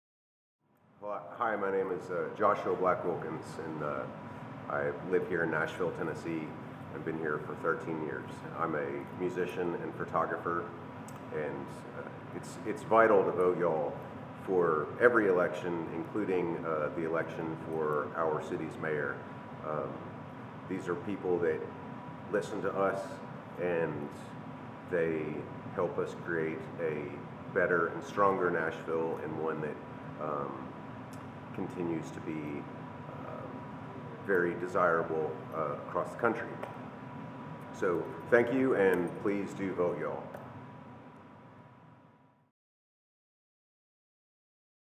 Joshua Black Wilkins' VoteYall video
Joshua Black Wilkins explains why it's important to VoteYall in this summer's election for a new mayor, vice mayor and Metro Council in Nashville.

Learn more about the race and become part of the VoteYall campaign